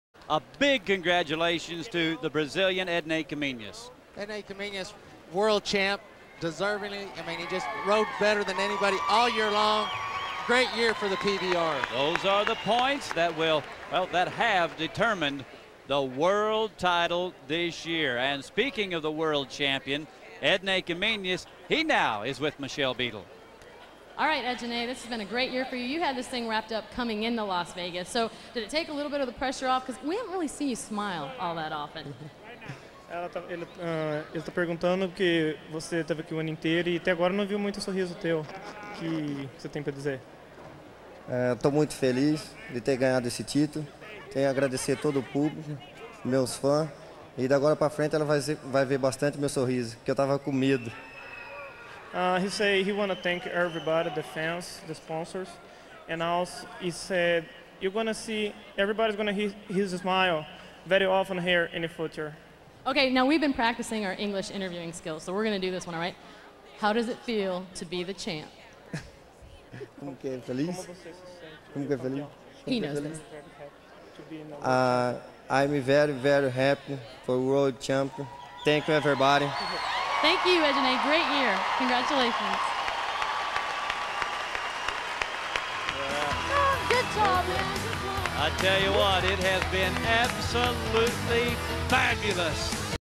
PBR 2002: Ednei Caminhas World Champion interview
TNN's Michelle Beadle speaks to the recently crowned PBR World Champion Ednei Caminhas after the Short-go of the 2002 World Finals. Ednei deserved that title. He rode more bulls that year than most could in their lifetimes.